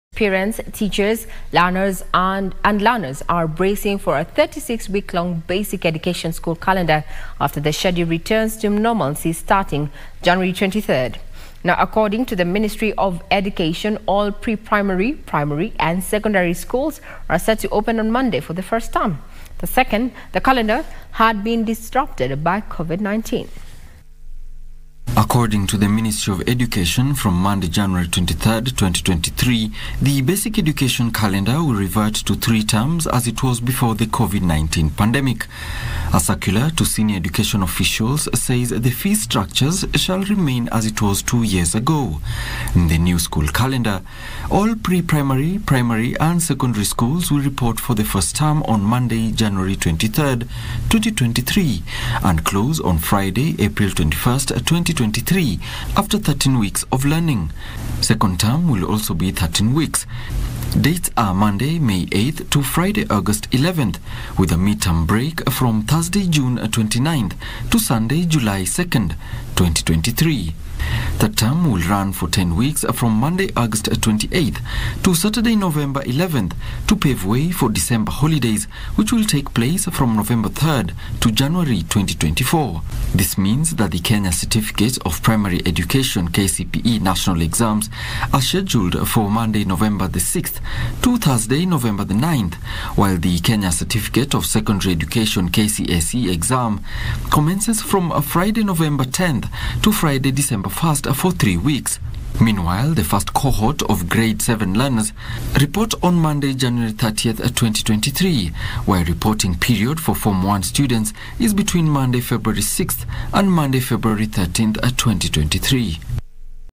Schools to re-open for the first term Monday January 23, 2023
Parents, teachers and learners are bracing for a 36-week-long basic education school calendar after the schedule returns to normalcy starting January 23, 2023. According to the Ministry of Education, all pre-primary, primary, and secondary schools are set to open on Monday for the first term. The calendar had been disrupted by Covid-19.